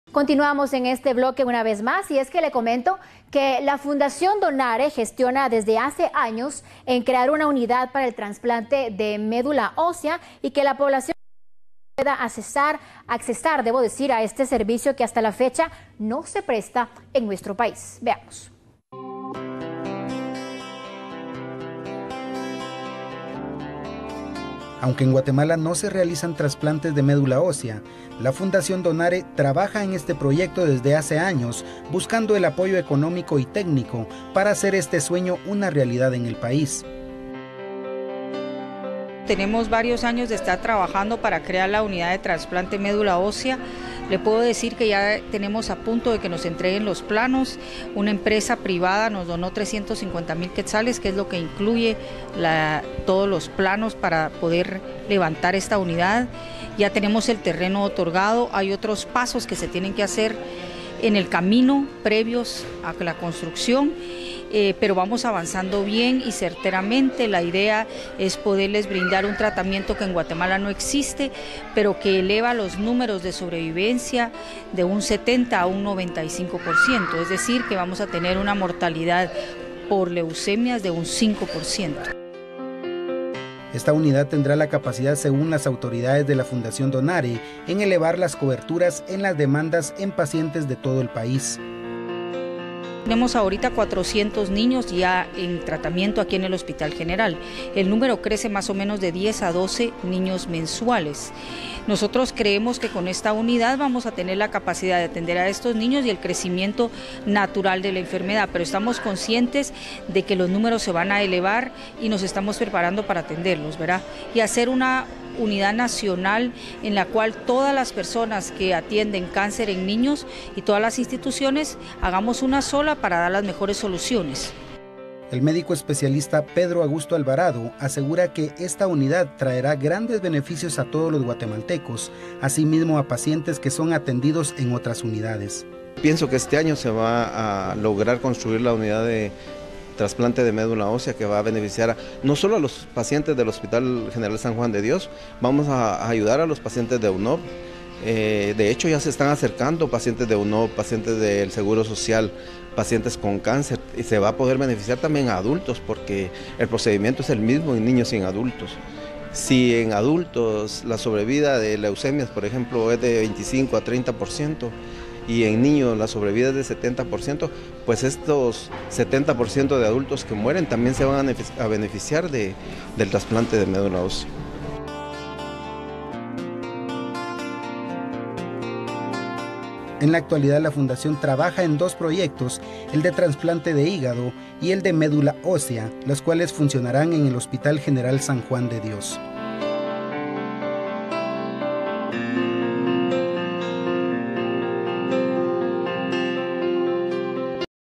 Buscan impulsar unidad de trasplante de médula osea
La iniciativa es gestionada durante años, por la Fundación Donare y pretender ayudar a la población a utilizar este servicio